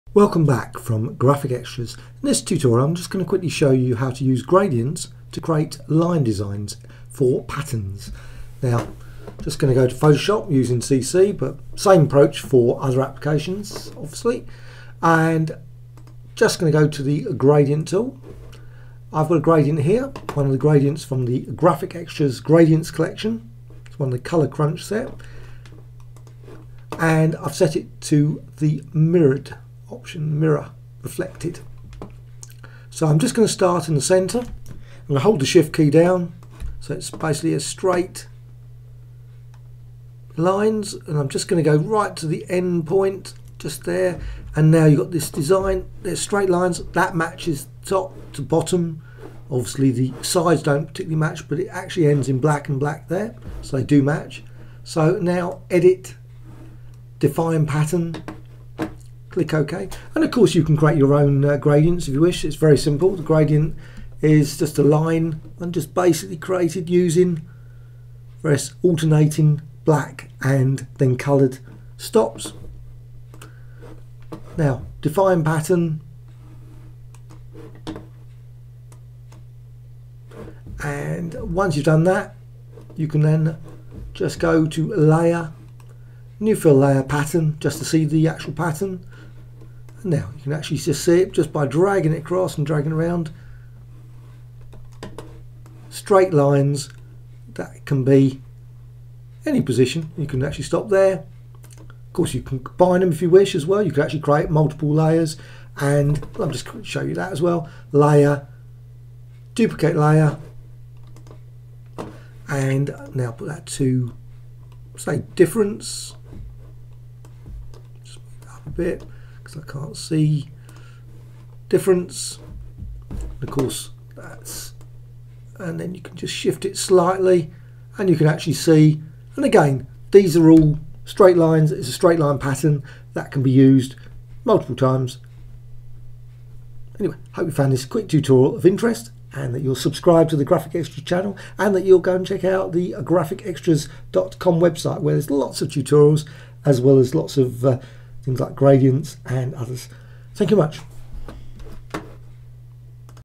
Photoshop CC tutorial : Create Photoshop line patterns using gradients. HOW TO
A guide to creating line patterns in Photoshop using  gradients (reflected mode) - you can create all kinds of line patterns in Photoshop CC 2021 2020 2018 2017 etc